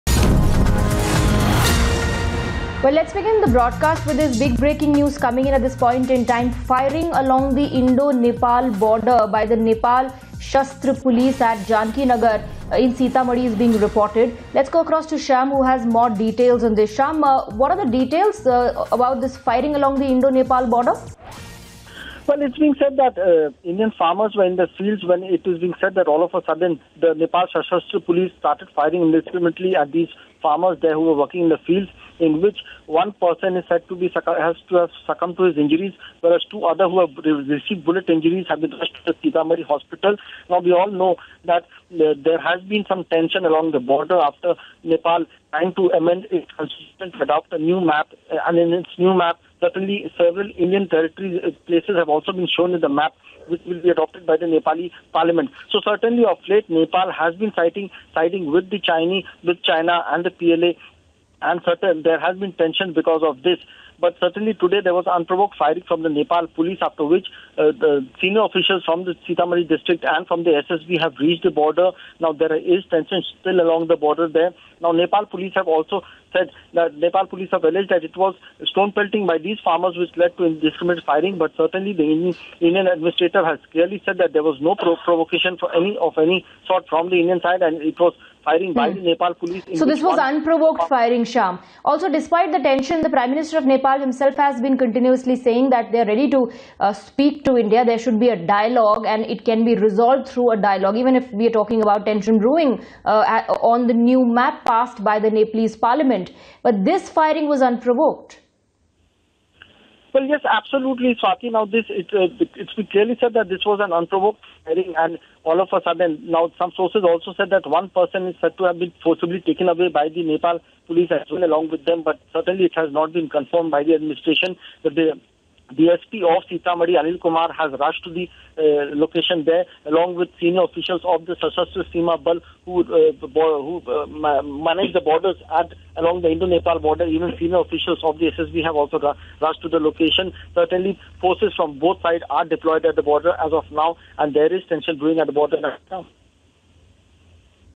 Firing by Nepal police reported along Indo-Nepal border; one farmer dead
Firing has been reported along Indo-Nepal border. Reportedly, Nepal Sashastra Police fired at farmers at Sitamarhi after which one farmer succumbed. Also, the incident occurred during the ongoing border dispute between India and Nepal. Both the injured Uday Thakur and Umesh Ram were rushed to Sitamarhi Sadar Hospital for immediate medical attention.
The area where the incident took place is between Janaki Nagar of Sitamarhi district and Sarlahi in Nepal.
Times Now has accessed FIRST VISUALS of the incident.
Know more.